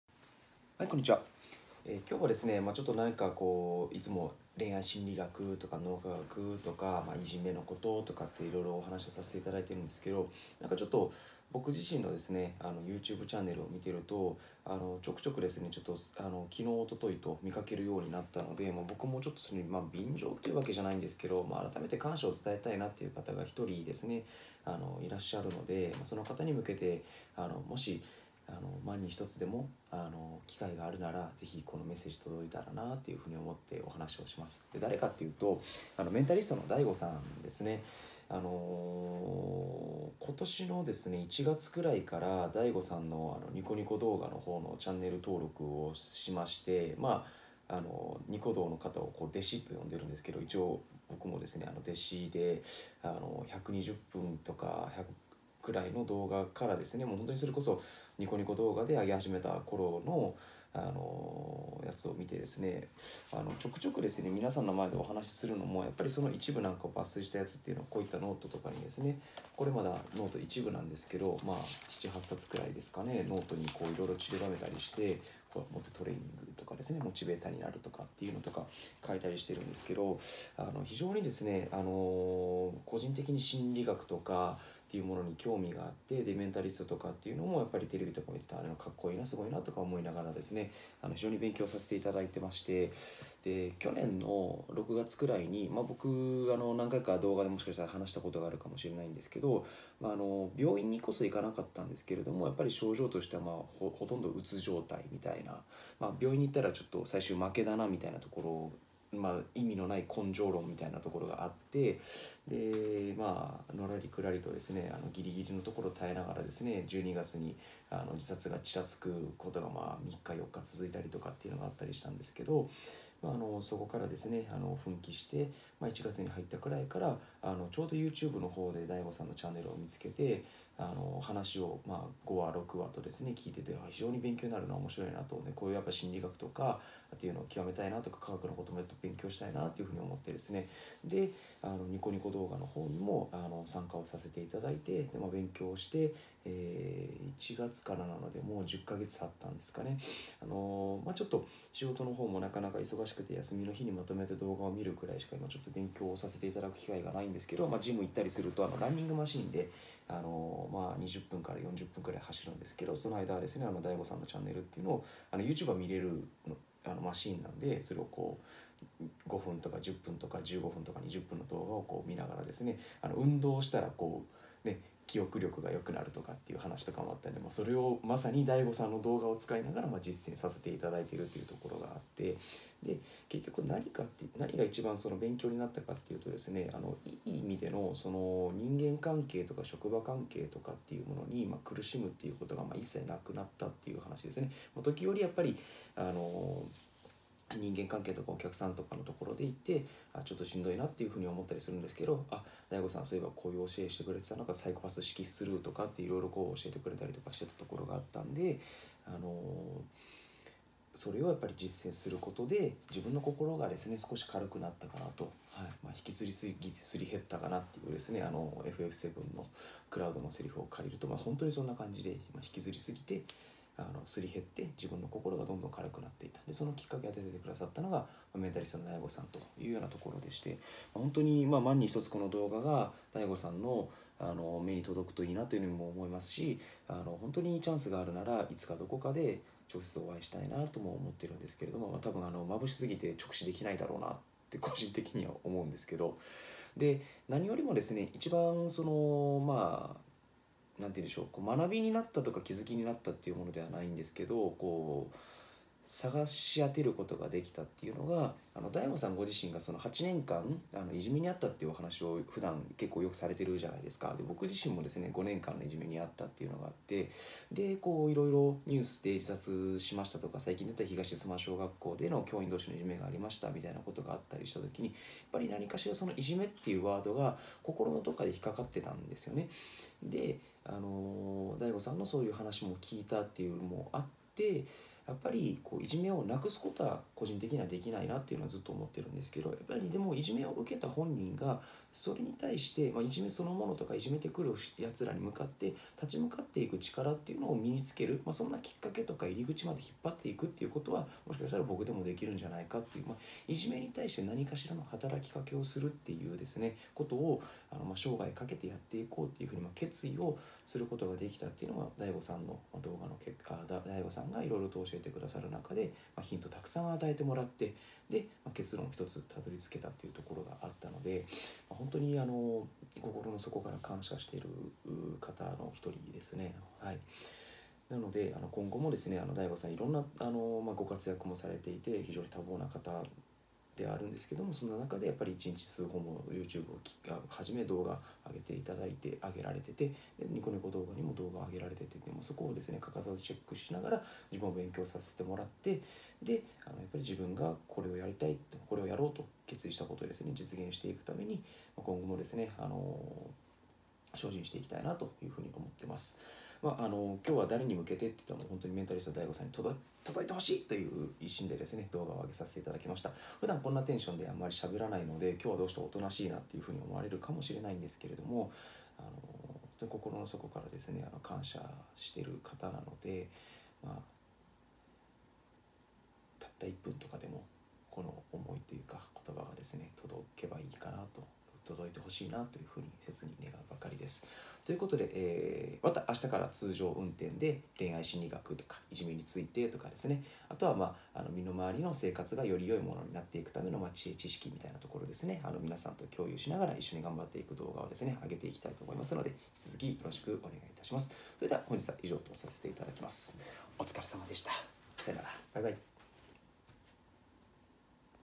【メンタリストDaiGoさん】に感謝。10ヶ月学んで心が軽くなった件と生涯かけて携わりたいことが見つかった件
1月からメンタリストDaiGoさんから学び続けた結果、人生が大きく変わりました。
ただ感謝、と、これからについて話している動画です。
この気持ちが届きますように。